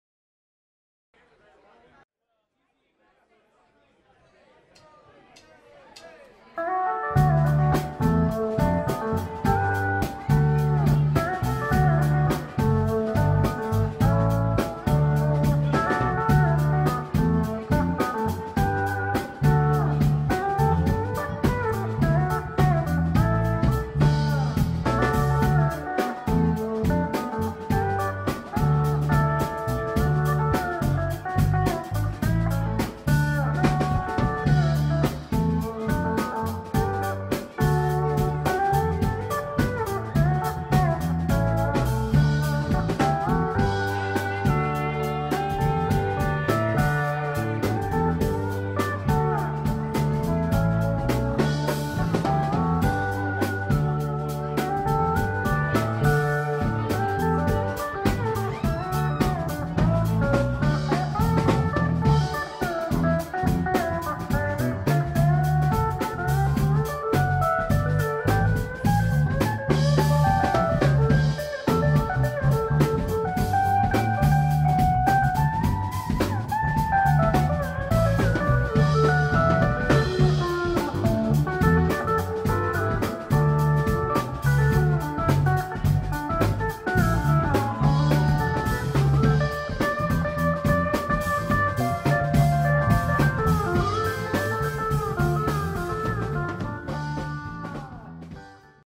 Gordon Stone
Short photo montage of the Gordon Stone Band with audio from show at Nectar's, Burlington, VT, December, 2008